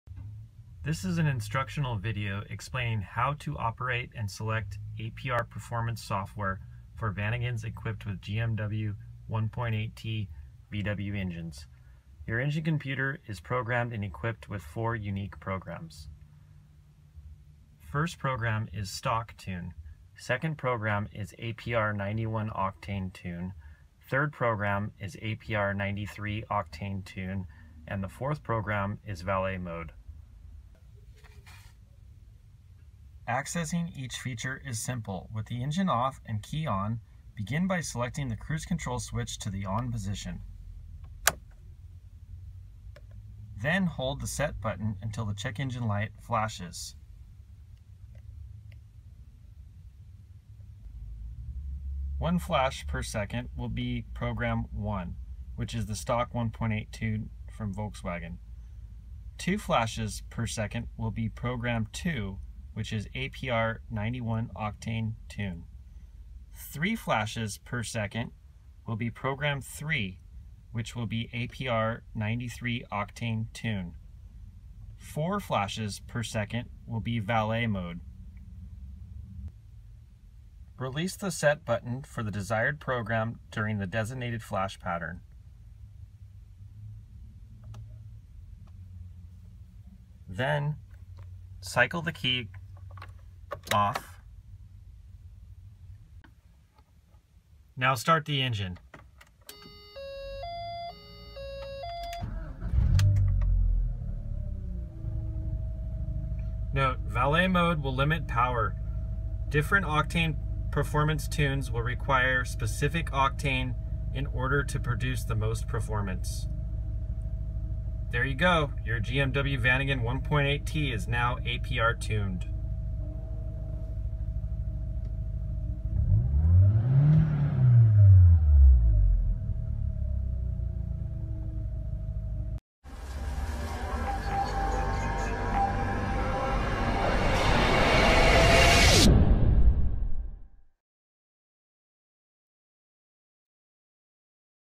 Vanagon 1.8T APR Performance Software Program Switching
Instructional video for German Motorwerks (GMW) Vanagon 1.8T engine
conversions equipped with our APR performance software (chip) option.